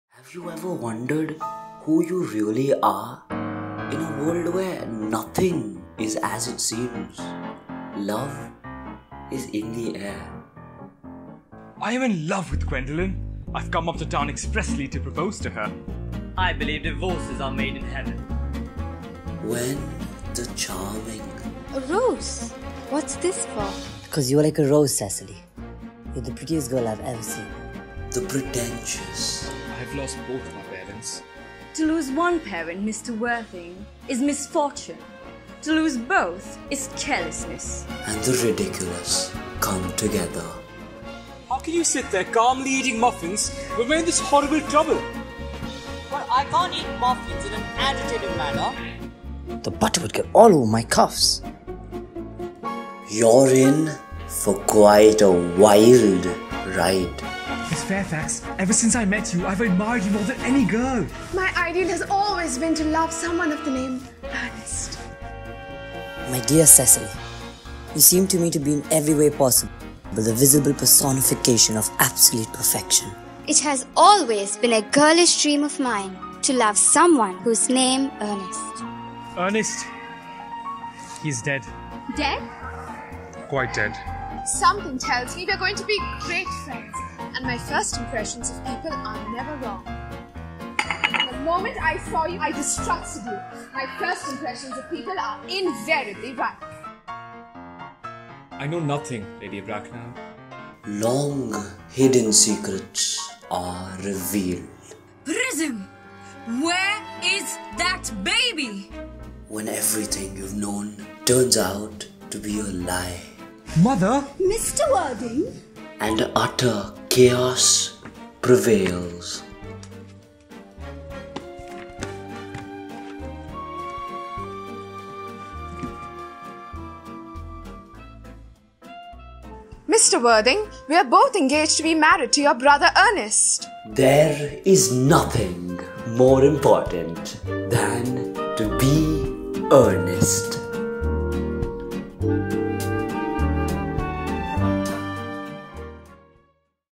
The Importance of Being Earnest Trailer | Ithaka'19
Have you ever wondered who you really are? What if you realised that all your life, you've been speaking nothing but the truth? In this "Trivial Comedy for Serious People", join Mr. Worthing and his equally eccentric friends, as they fall in passionate love, have ground-breaking epiphanies and explore the curious case of the Handbag, all in the course of one ridiculously eventful day. As façades crumble, will the truth finally come out?
Ithaka 2019 presents Oscar Wilde’s The Importance of Being Earnest, directed by Sharanya Kumar and Juie Gune.
9th December: 5 p.m. to 6 p.m.
10th December: 2 p.m. to 3 p.m.
Venue: College Hall.